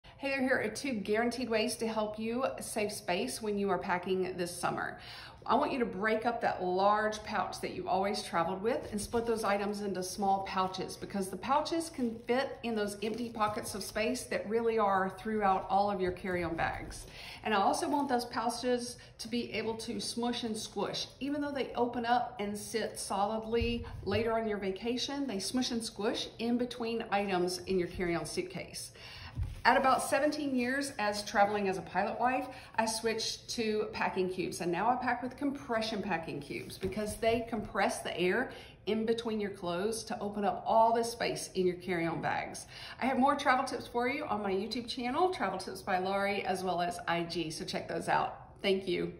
2 Ways to Save Space in Carry-On Bags (Summer)
Here are 2 ways to save space in carry-on bags this summer. When you travel, break the large pouch into smaller pouches because they can fit into empty pockets of space in your carry-on suitcase. Pack with pouches that can squeeze inbetween travel items. I pack with compression packing cubes because that opens up extra space in all of your carry-on bags.

Hi everyone. This is a Shorts video with travel tips. I am testing it.

POP over and say hi:
Instagram:  @TravelTipsByLaurie (My former channel name was LauriePOP Ideas That POP, but now I produce travel tip videos only)